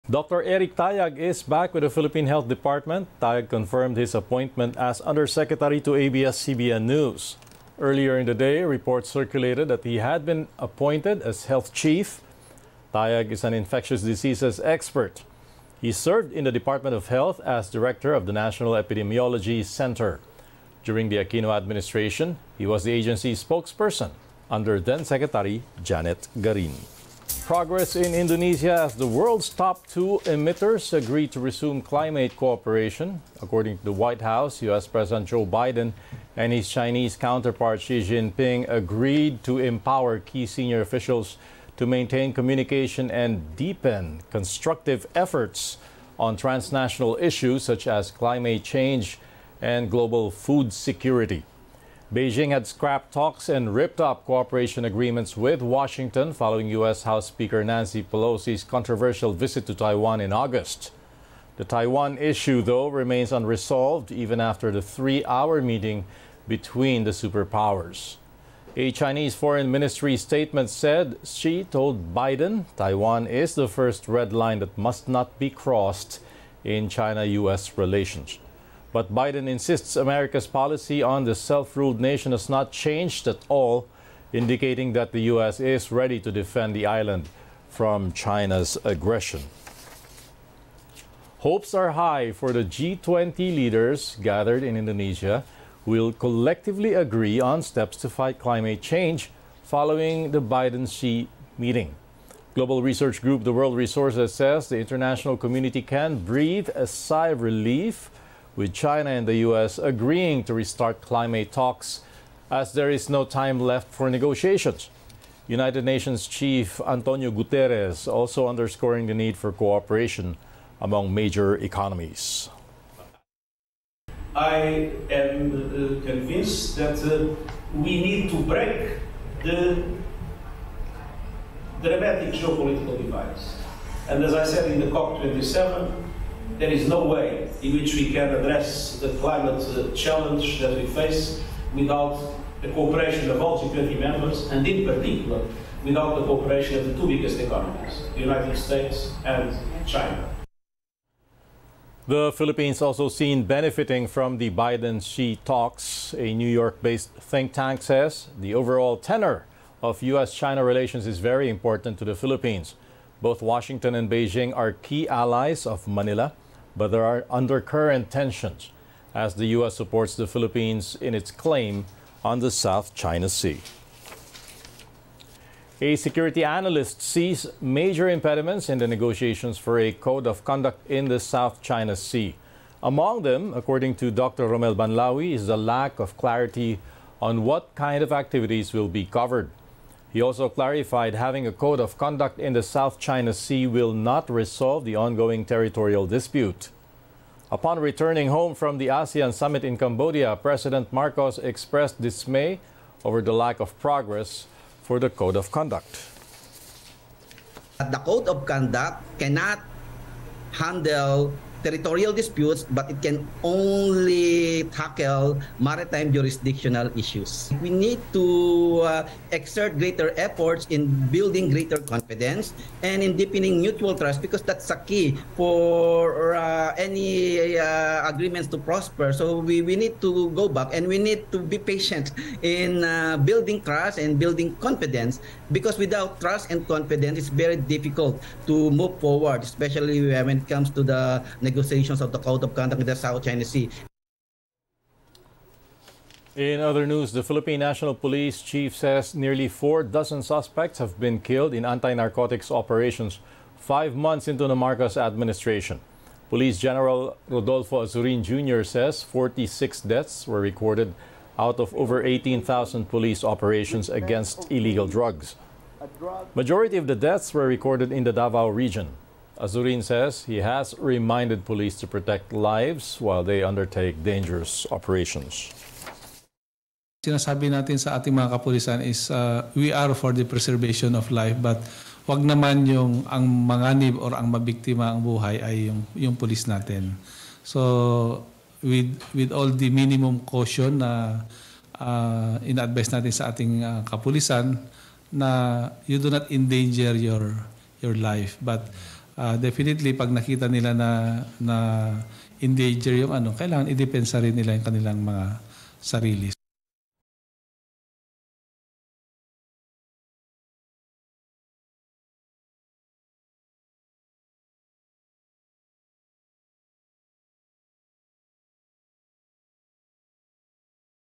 Bongbong Marcos appoints Dr. Eric Tayag as DOH Undersecretary | ANC
Rundown: Dr. Eric Tayag is back in DOH.